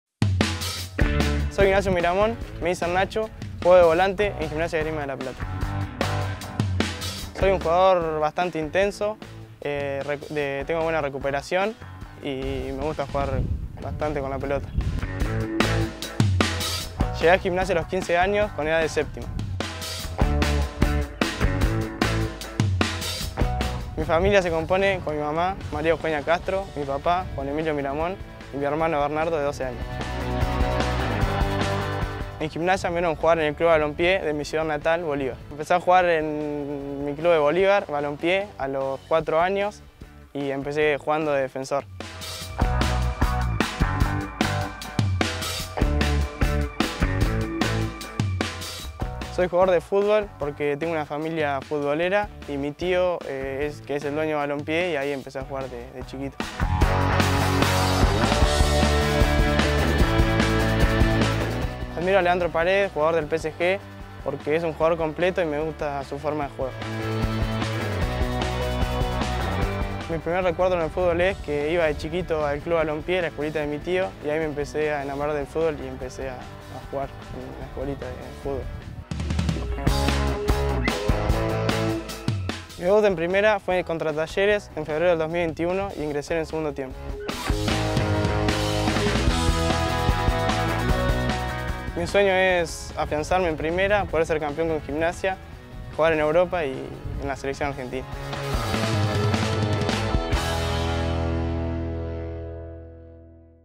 Nacho Miramón: "Mi sueño es afianzarme en Primera y ser campeón con Gimnasia"
Conocé al mediocampista Ignacio Miramón, fundamental en el equipo de la ReservaLPF de Gimnasia. Arrancó jugando de defensor y ahora es uno de los volantes centrales más destacados de la categoría. ¿A quién admira? a Leo Paredes 5⃣